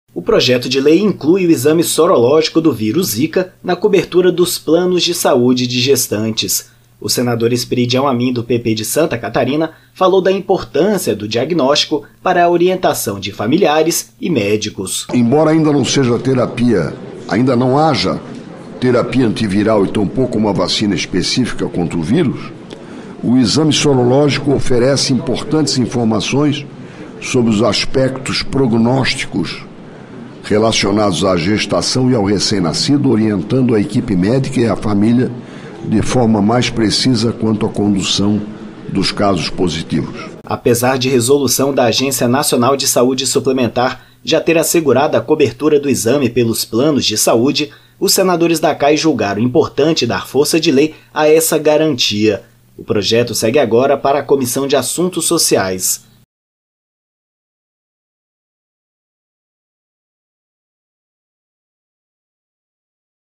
Senado discute cobertura do teste do zika vírus para gestantes pelos planos de saúde
A Comissão de Assuntos Econômicos aprovou na terça-feira (16/05) o projeto de lei PL 1.855/2019, que obriga os planos de saúde a cobrir o teste sorológico para zika vírus para pacientes grávidas. O texto do senador Weverton (PDT-MA) segue para a Comissão de Assuntos Sociais (CAS).